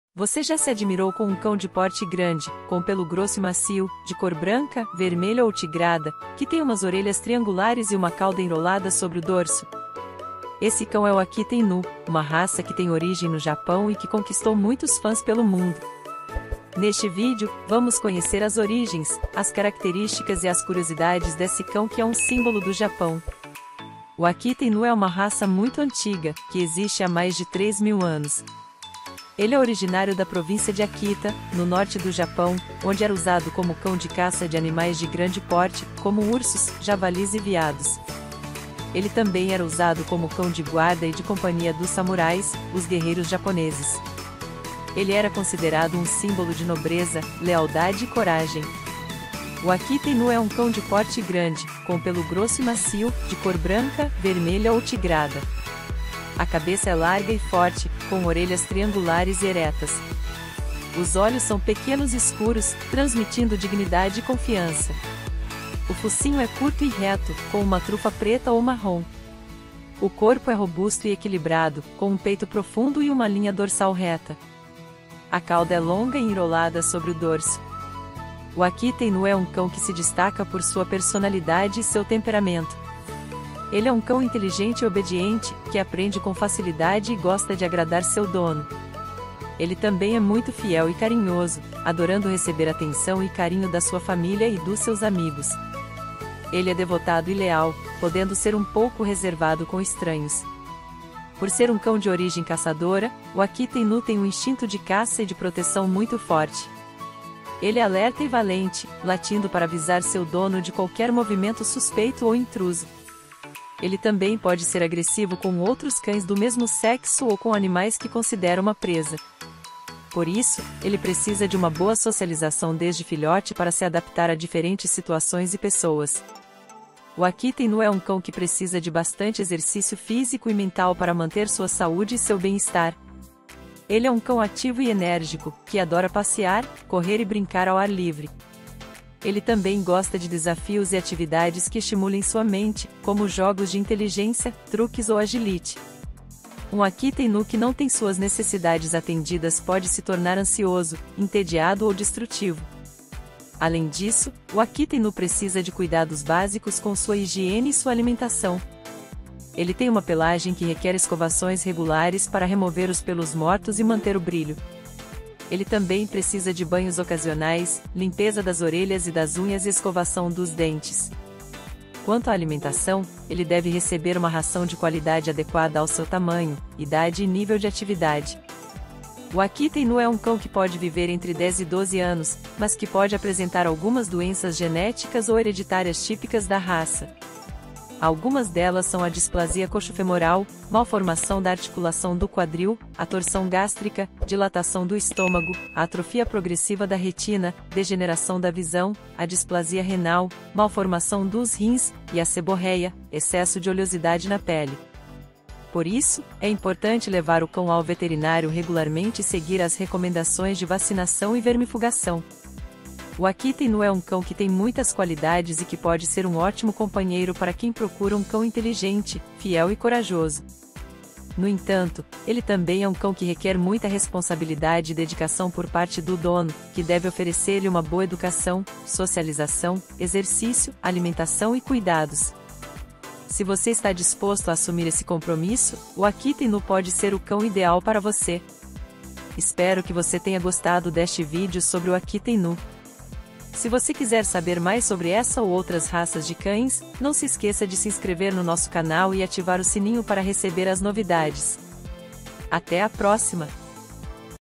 Akita Inu: o cão que é um símbolo do Japão!
Sabia que o Akita Inu é um cão que é um símbolo do Japão? Que ele tem uma origem milenar, sendo usado como cão de caça, de guarda e de companhia dos samurais? Que ele é um cão inteligente, fiel e corajoso, que adora proteger sua família e seus amigos? Neste vídeo, você vai descobrir tudo isso e muito mais sobre esse cãozinho que é um fenômeno!

É uma raça muito antiga, que existe há mais de 3000 anos. É originário da província de Akita, no norte do Japão, onde era usado como cão pastor e de guarda nas fazendas. Era capaz de conduzir o gado mordendo seus calcanhares e de proteger as propriedades de intrusos e predadores. Ele também era um companheiro fiel dos camponeses e dos nobres, sendo muito apreciado pela família real britânica.

É um cão de porte grande, com pelo grosso e macio, de cor branca, vermelha ou tigrada. A cabeça é larga e forte, com orelhas triangulares e eretas. Os olhos são pequenos e escuros, transmitindo dignidade e confiança. O focinho é curto e reto, com uma trufa preta ou marrom. O corpo é robusto e equilibrado, com um peito profundo e uma linha dorsal reta. A cauda é longa e enrolada sobre o dorso.

O Akita Inu é um cão que tem uma personalidade e um temperamento marcantes. É inteligente e obediente, aprendendo com facilidade e gostando de agradar seu dono. Ele também é muito fiel e carinhoso, adorando receber atenção e carinho da sua família e dos seus amigos. É devotado e leal, podendo ser um pouco reservado com estranhos.

Neste vídeo, você vai conhecer as origens, as características e as curiosidades do Akita Inu, um cão que tem muitas qualidades e que pode ser um ótimo companheiro para quem procura um cão inteligente, fiel e corajoso.